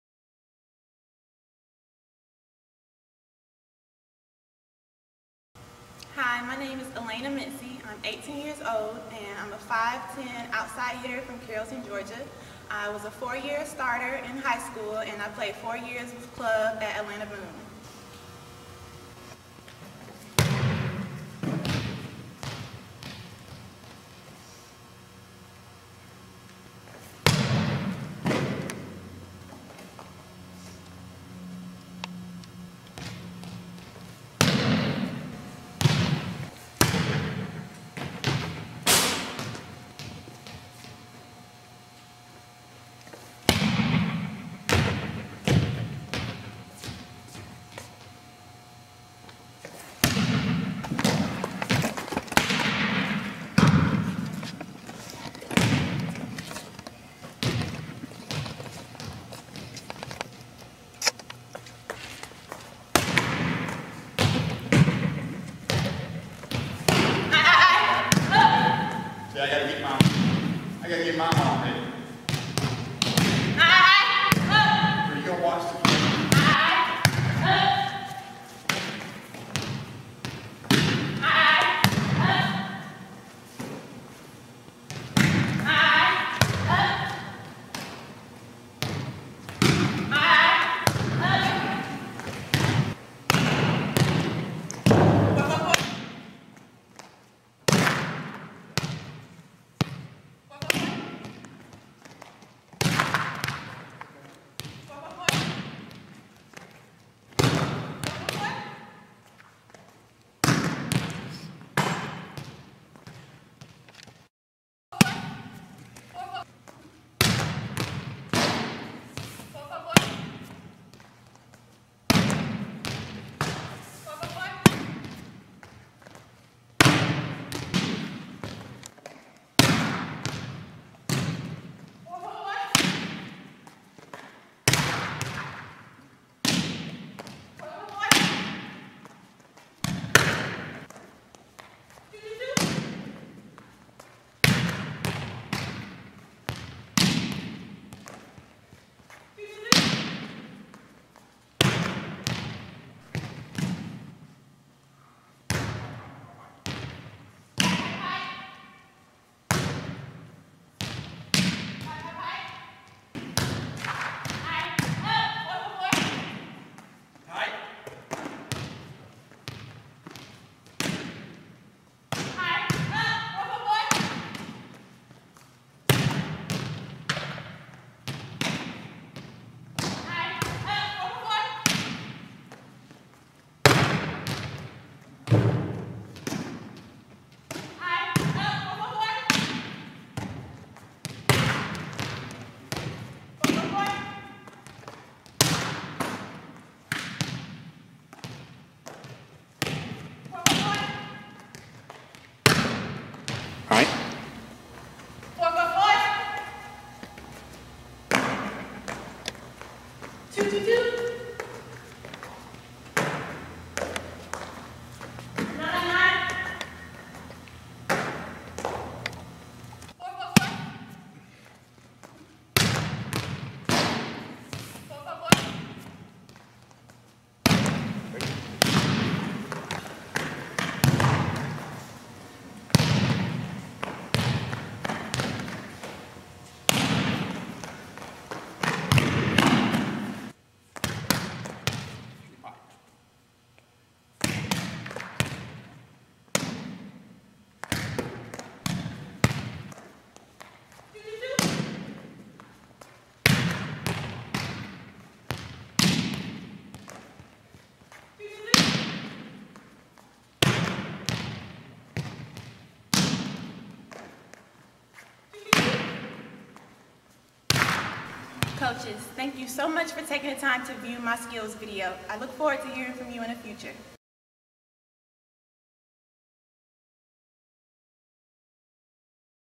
Elena Mincy Volleyball Skills Video
Elena Mincy showcases her volleyball skills.